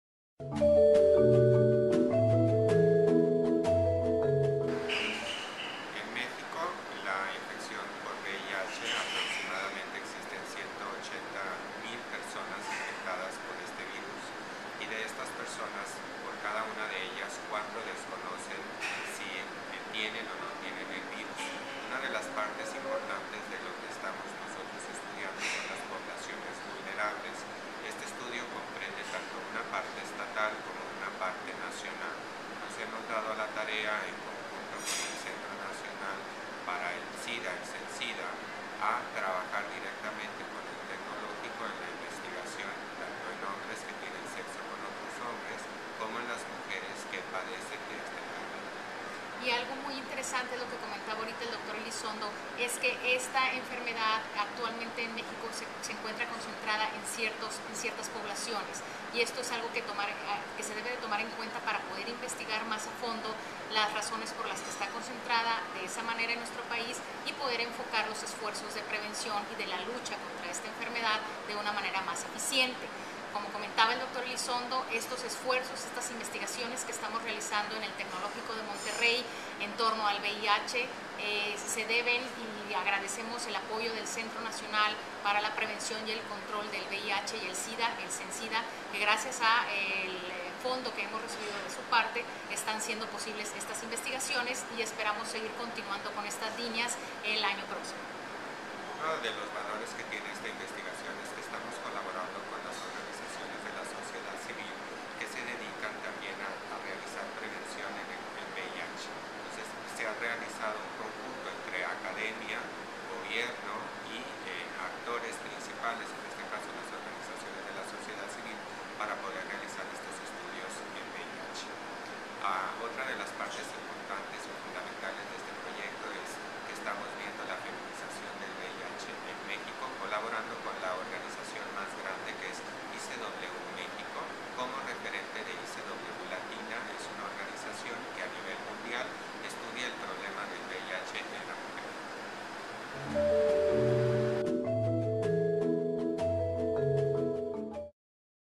Cápsula Informativa: Datos sobre investigación del VIH SIDA
Dr. Jesus Eduardo Elizondo y Dra. Ana Cecilia Treviño, Investigadores en temas de Vih Sida.